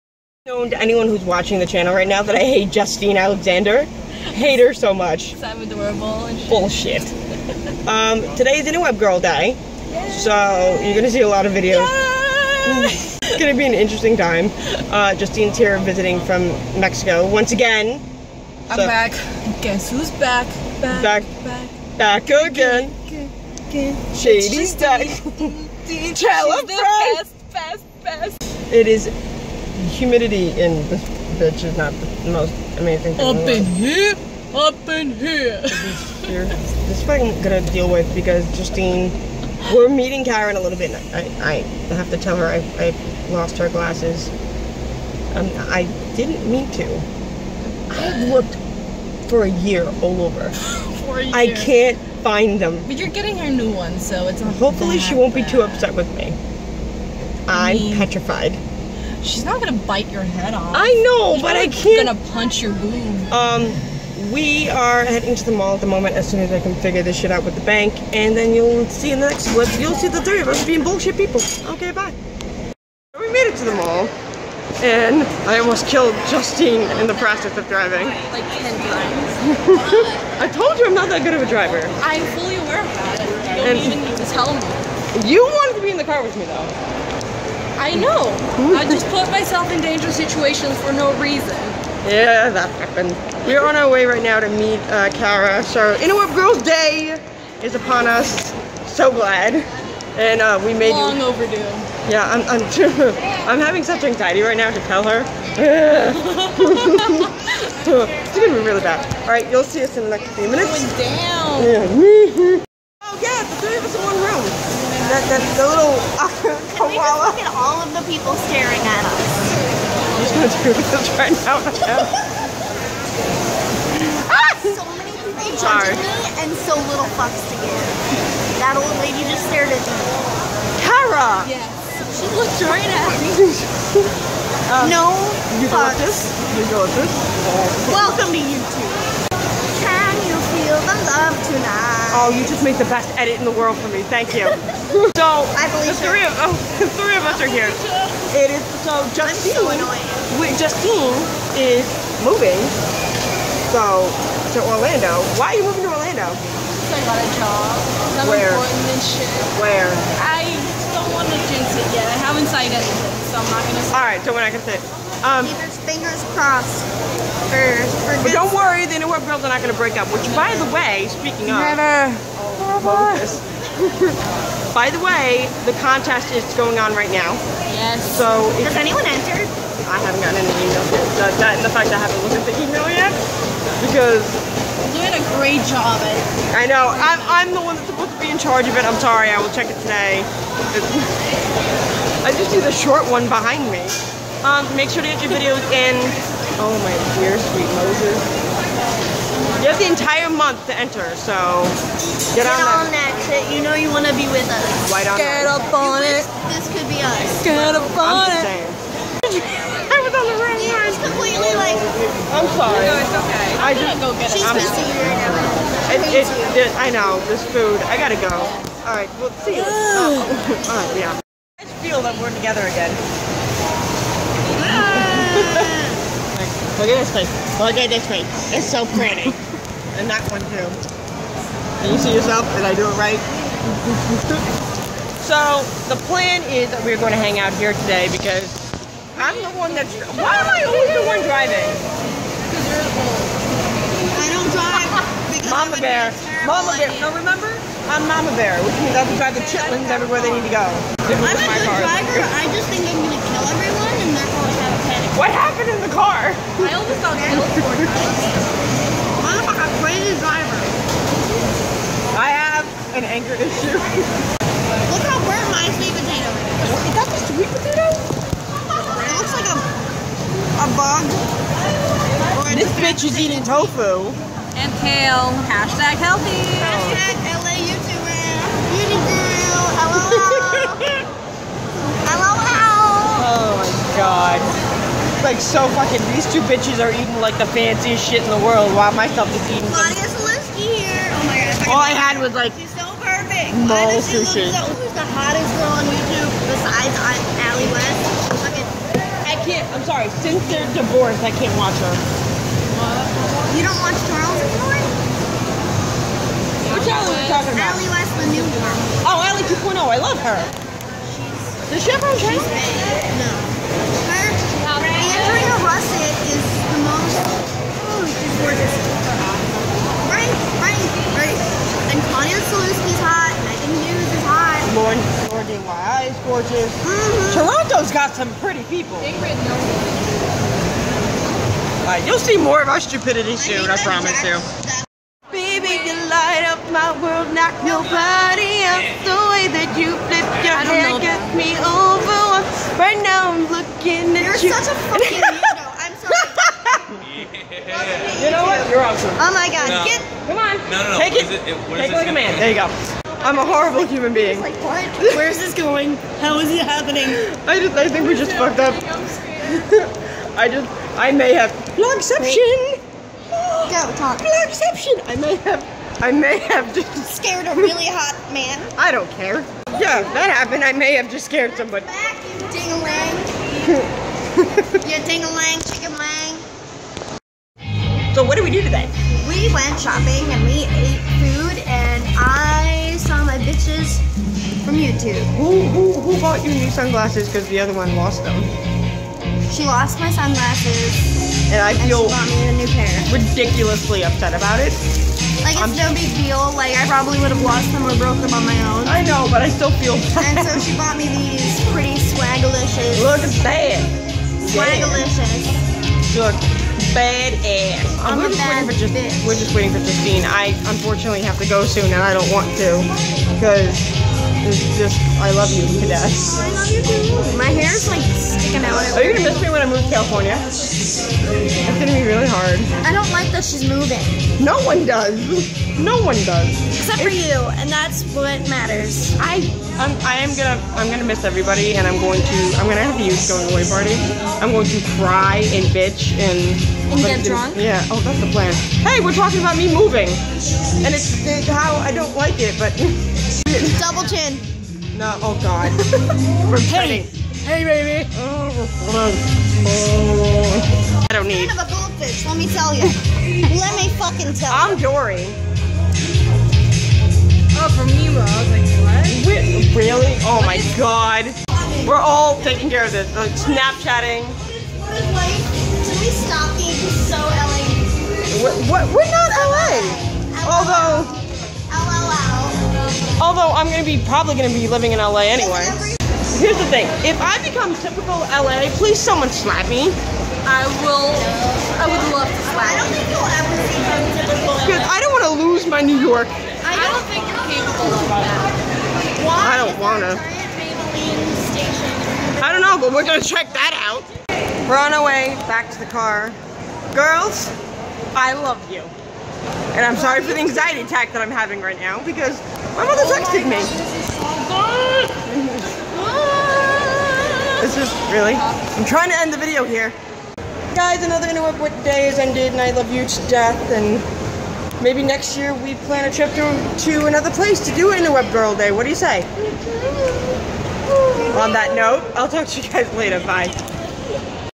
the return of theinterwebgirls day !
the annual theinterwebgirls day is back! this is what happens when you put me Justine and kara in a room for too long lol